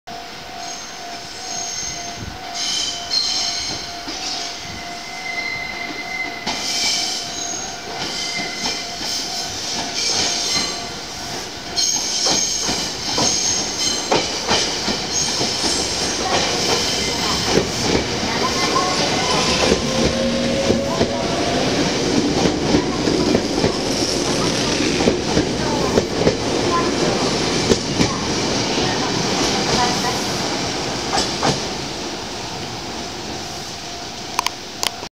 名鉄東岡崎駅普通岩倉行き到着
名鉄東岡崎駅にて撮影。普通岩倉行きが到着します。